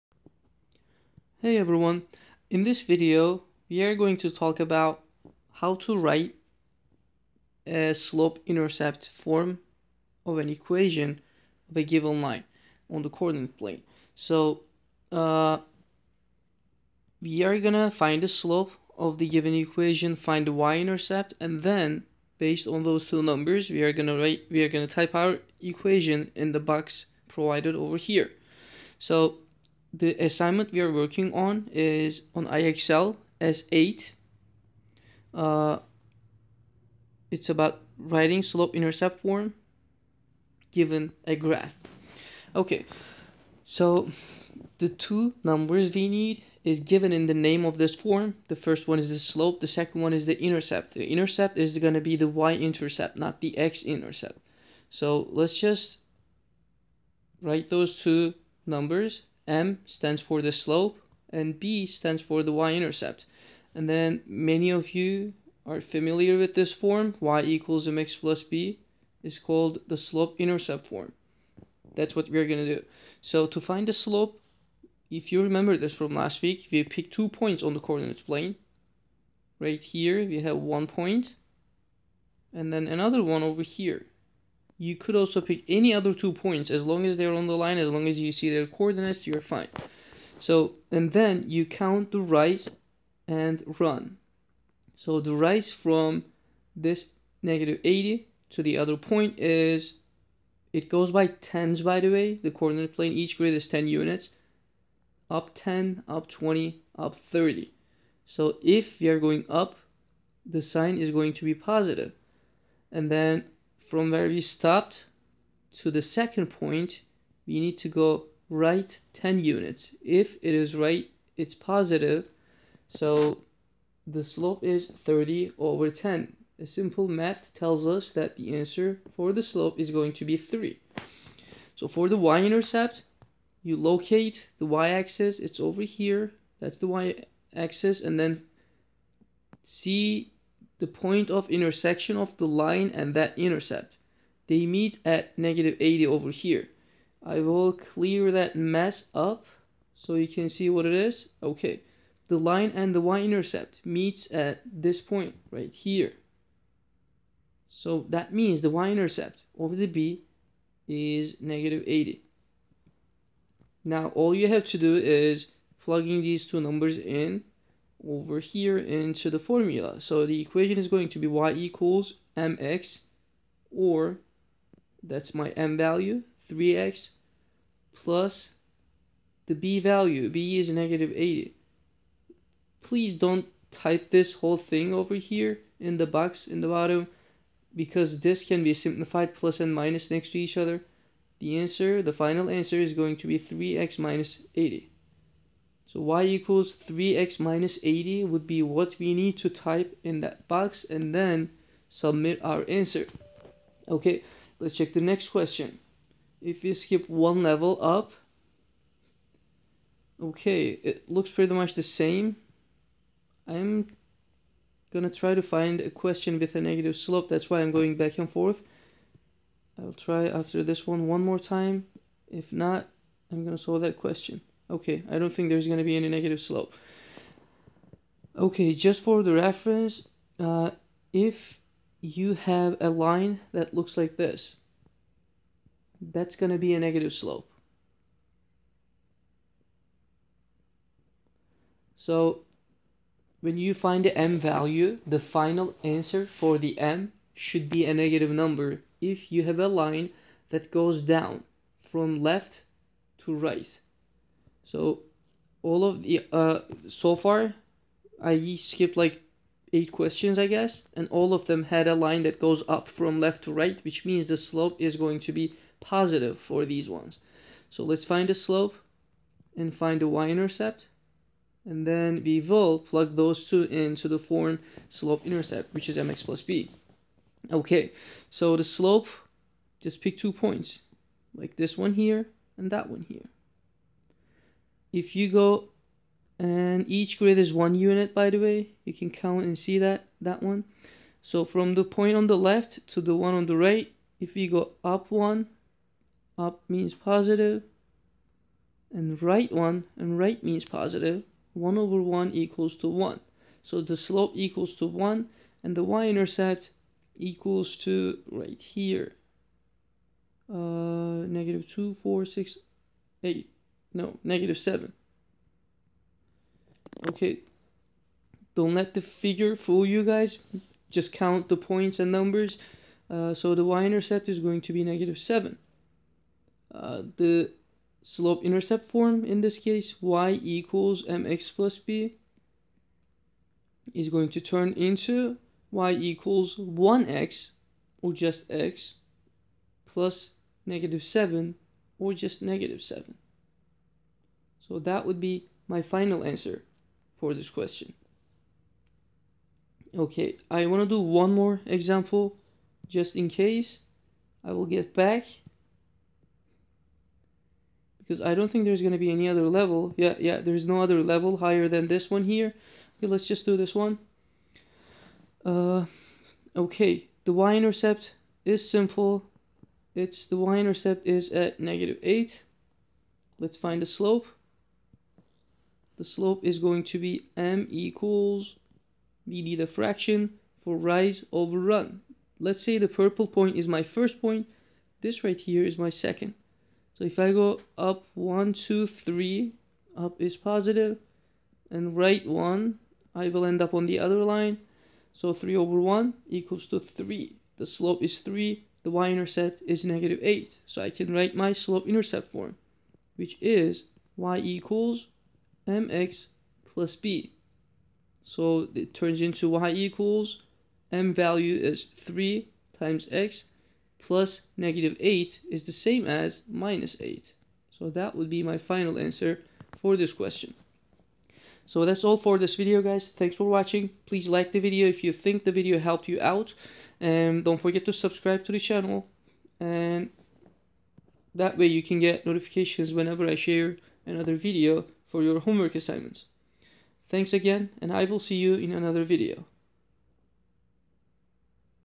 IXL S.8 Slope-intercept form: write an equation from a graph
This video shows examples of how to write an equation in slope-intercept form when a graph is given.